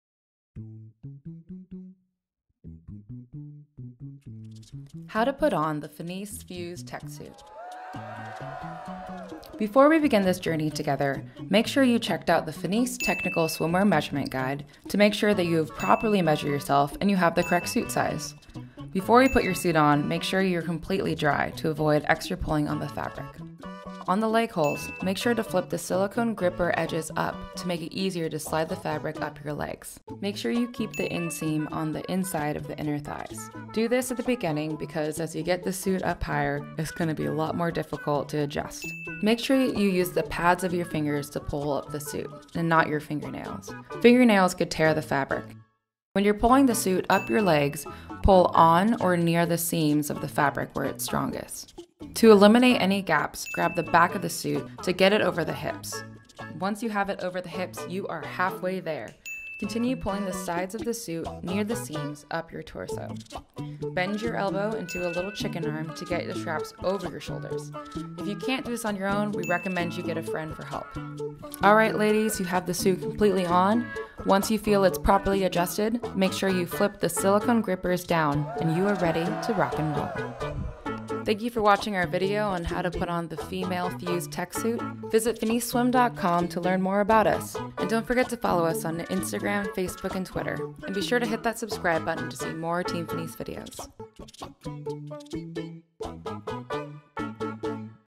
How To Put On FINIS Female Fuse Tech Suit
Here's some tips into getting into a FINIS Female Fuse Tech Suit!